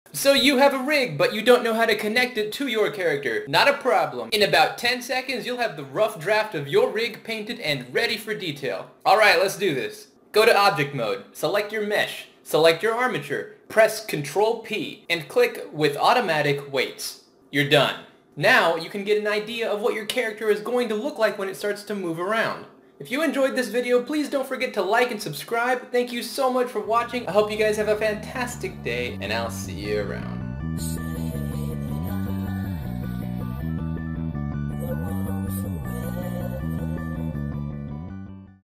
Blender 2.8 Automatic Weight Paint In 10 Seconds!
Hi everyone! In about 10 seconds you'll understand everything you need to know about Blender 2.8's automatic weight paint feature!

It took over 3 years to create and has thousands of hours and heart put into it :)

-Royal Skies-